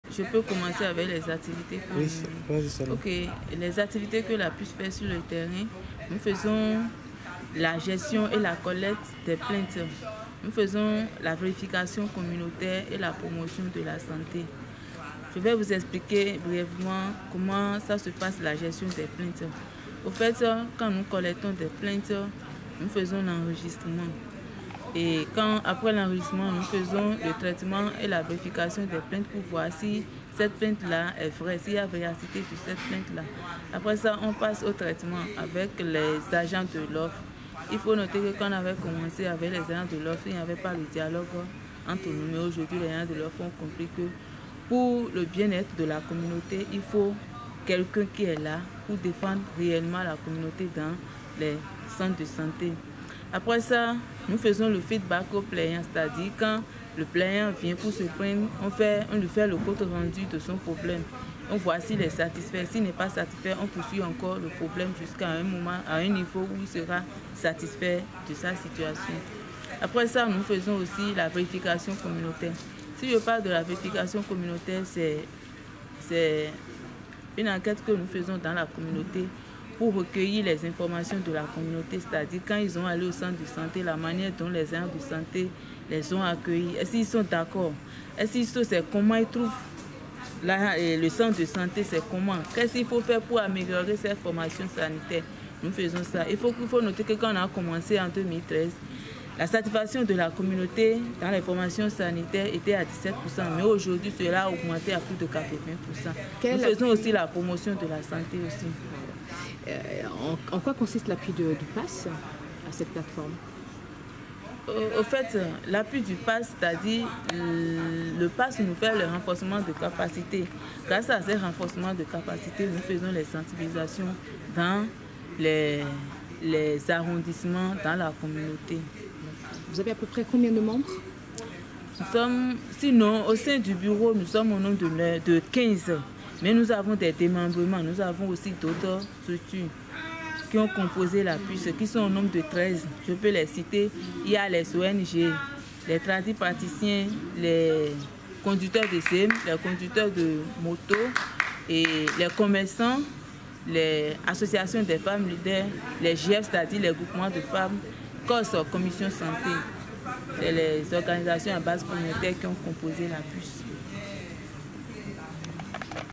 CTB BENIN | Les activités du PUSS KTL
Les PUSS, sont les Plateformes des Utilisateurs des Services de Santé). La Présidente de la PUSS Klouekanmè, Toviklin et Lalo nous précise les activités menées sur le terrain au bénéfice des membres.